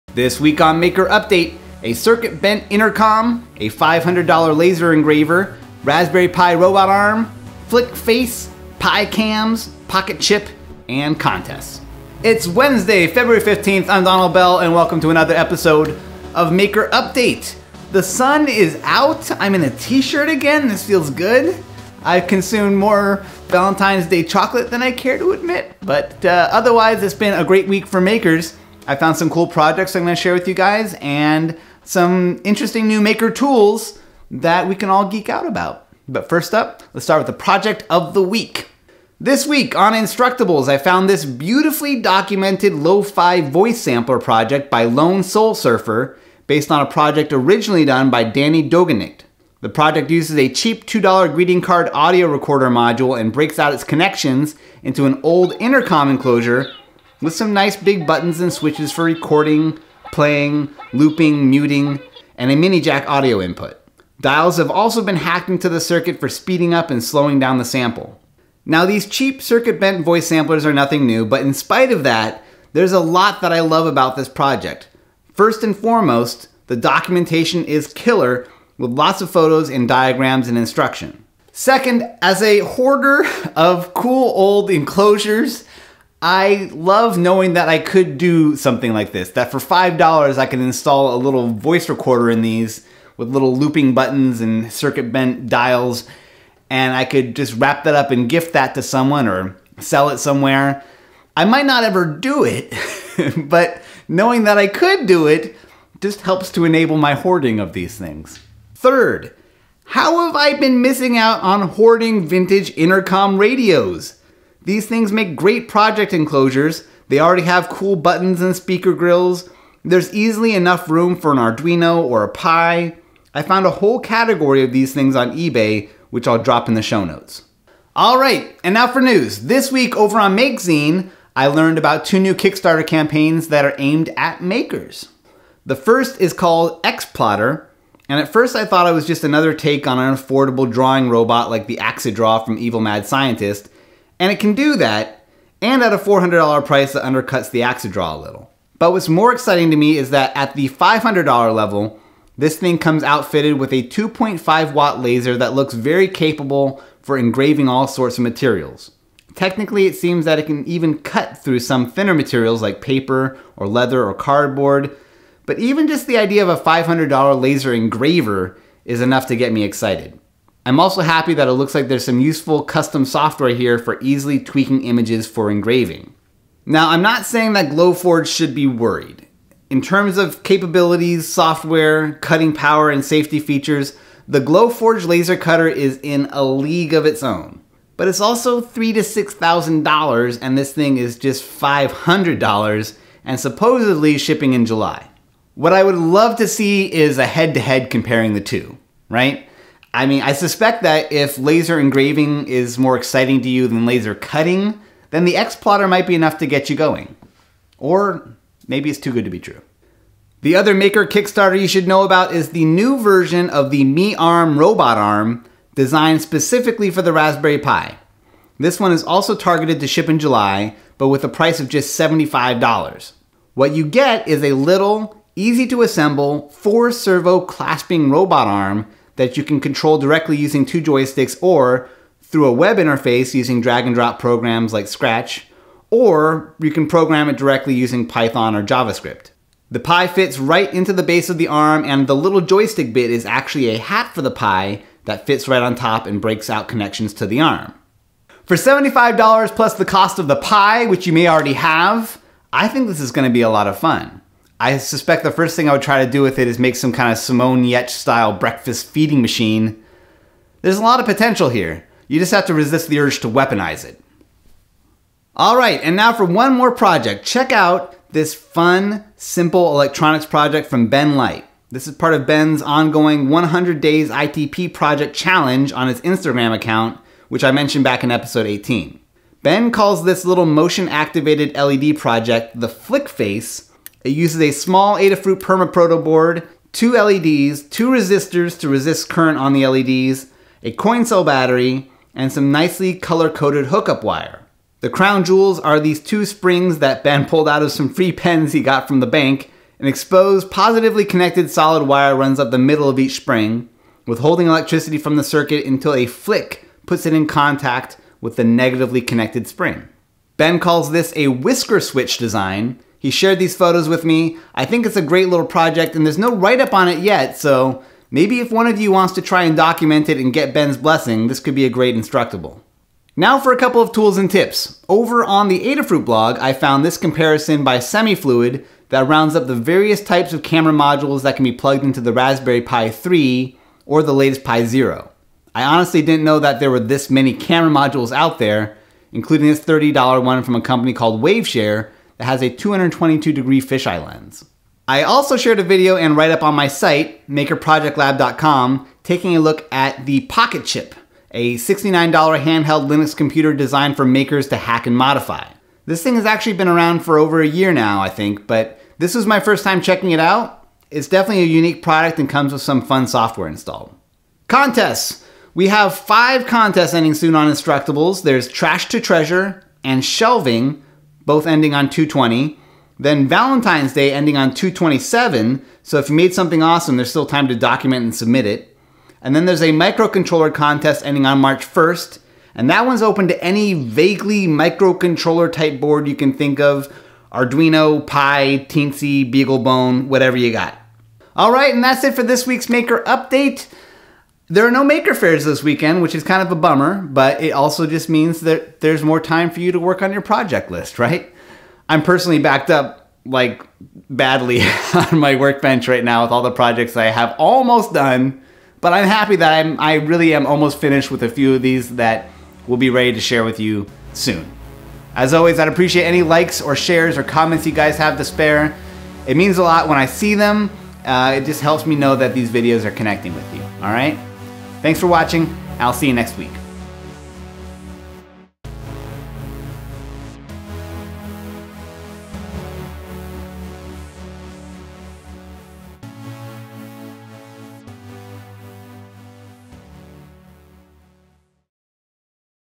Hacking a $2 Voice Recorder [Maker Update #21]
This week on Maker Update, a circuit-bent voice recording intercom, a $500 laser engraver, a Raspberry Pi robot arm, Flick Face electronics project, Pi Cams compared, a PocketCHIP review, and contests!

Project of the Week (0:38)
Lo-Fi Voice Sampler (lonesoulsurfer)
Cheap Recorder Module
Vintage Intercom Enclosures

News
$500 XPlotter Laser Engraver Kickstarter (2:15)
MeArm Pi Kickstarter (3:29)

Projects
Flick Face (Ben Light) (4:24)

Tools/Tips
Pi Cameras Compared (5:22)
PocketCHIP Review (5:44)

Contests Ending Soon (6:04)